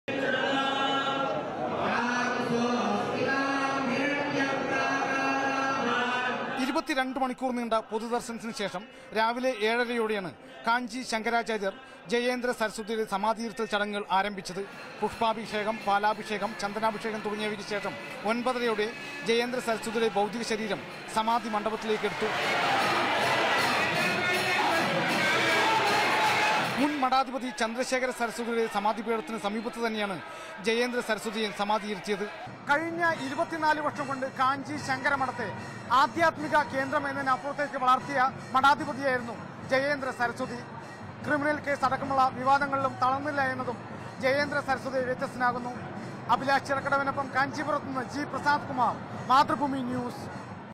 Kanchi Seer Jayendra Saraswathi Buried Next To His Guru| Mathrubhumi News
Chennai: The final rituals for burying the mortal remains of Kanchi seer Jayendra Saraswathi called 'Brindhavana Pravesam' have been completed. The rituals began with an 'abishekam' with milk and honey, followed by recital of Vedic hymns by priests in the presence of Sri Vijayendra Saraswathi, the junior pontiff of the Mutt and some close family members. Thousands of devotees and prominent persons like Tamil Nadu governor Banwarilal Purohit, union ministers Sadananda Gowda and Pon Radhakrishnan were in attendance.